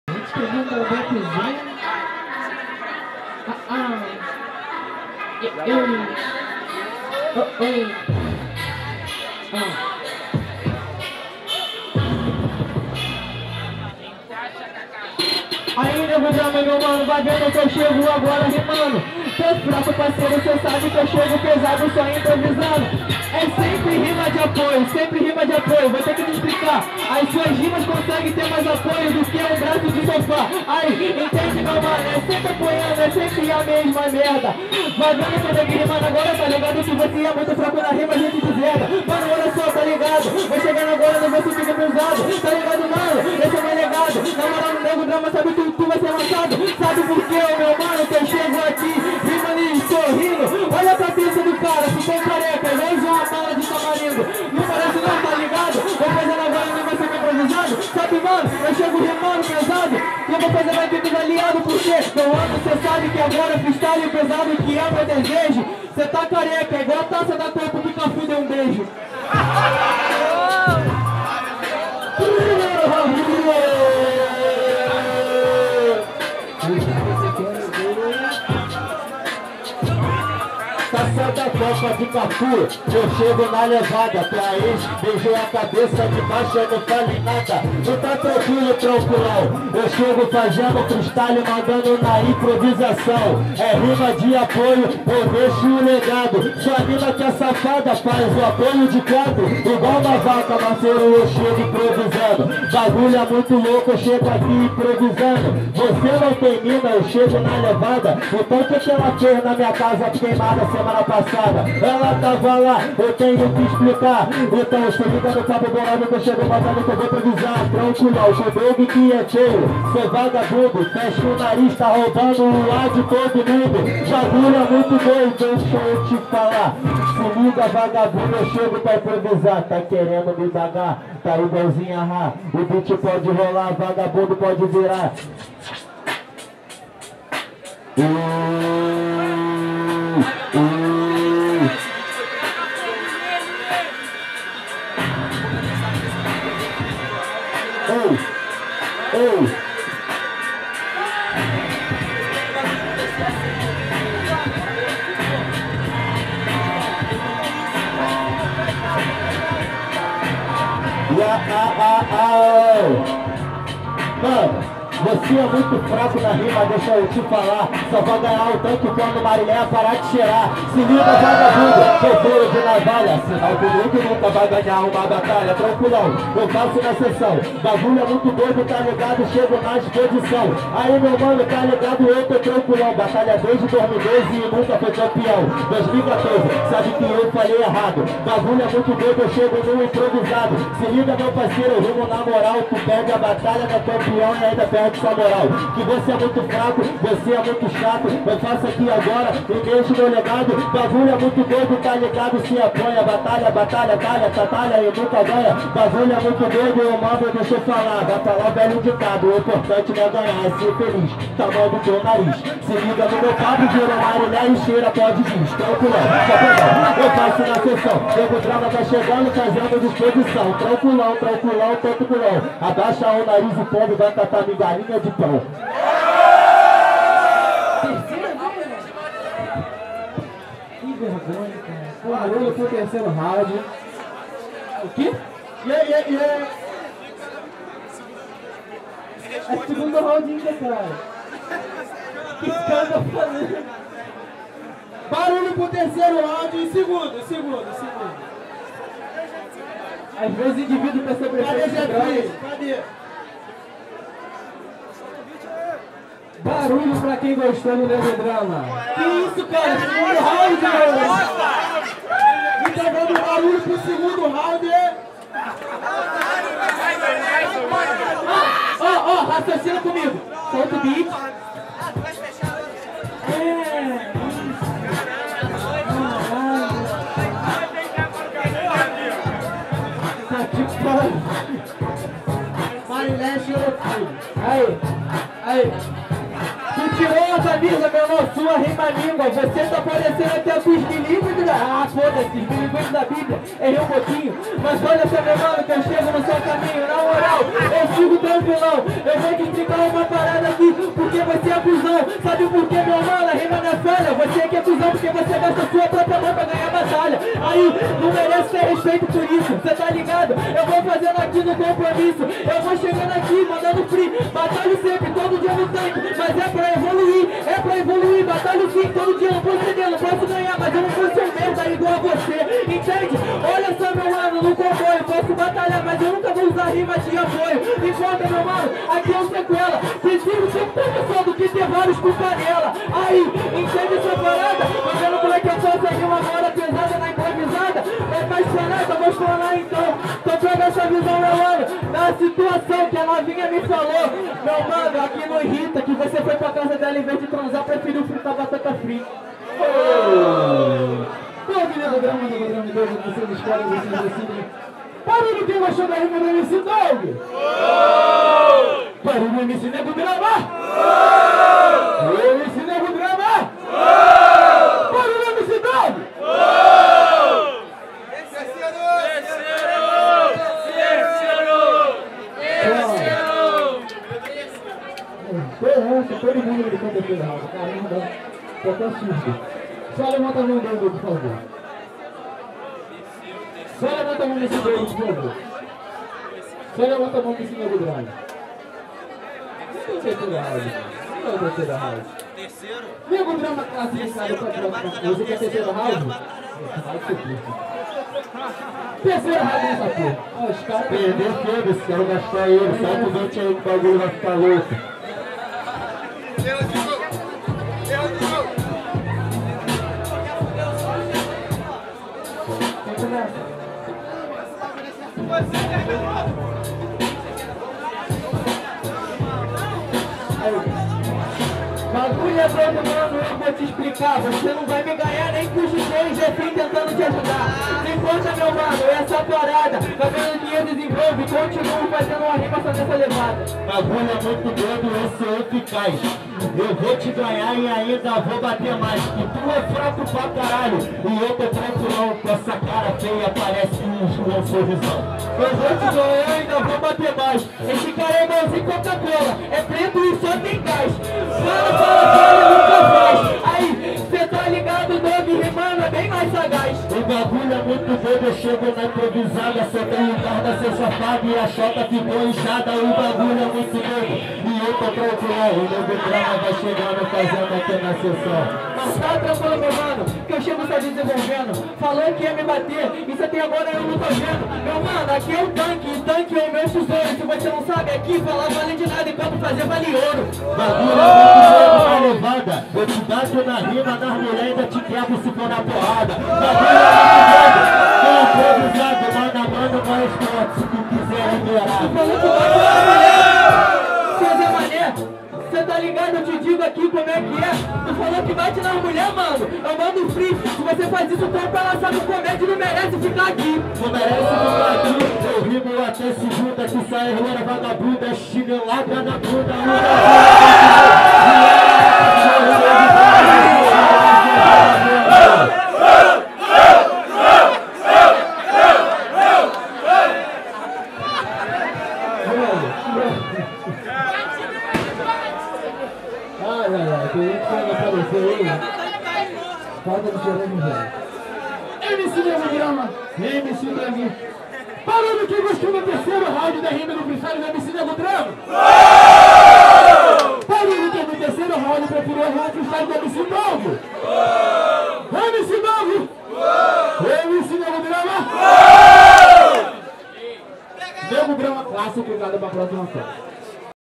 Doug vs Nego Drama - 234º Batalha do Tanque - São Gonçalo - 2017
Vencedor da Batalha: Nego Drama